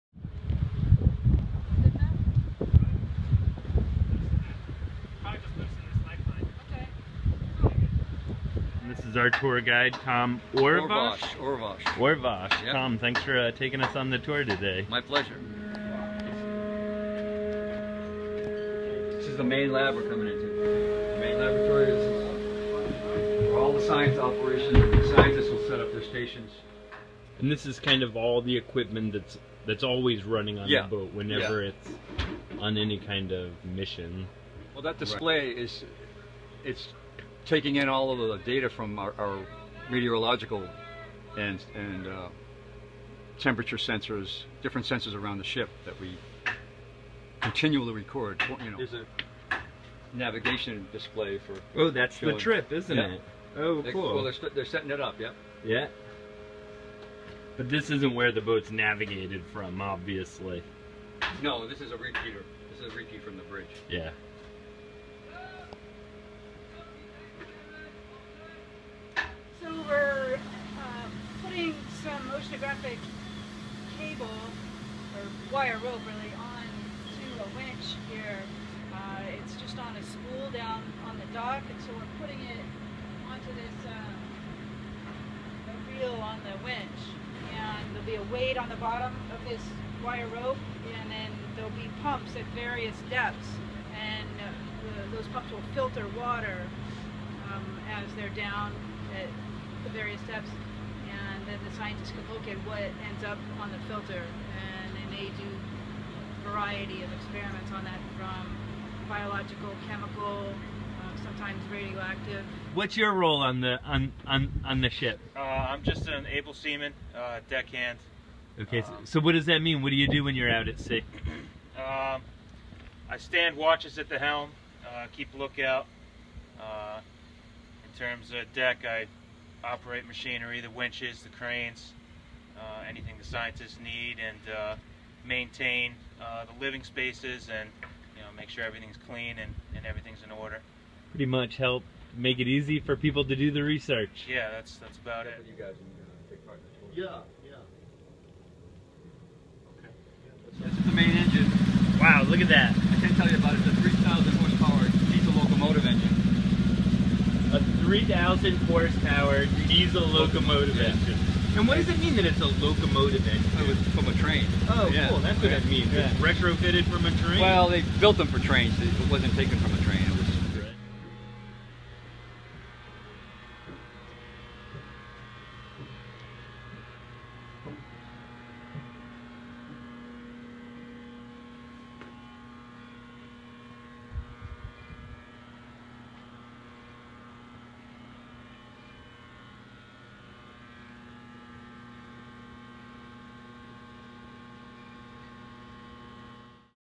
A virtual tour of the RV Endeavor
The RV Endeavor is a research vessel stationed at the URI Bay Campus.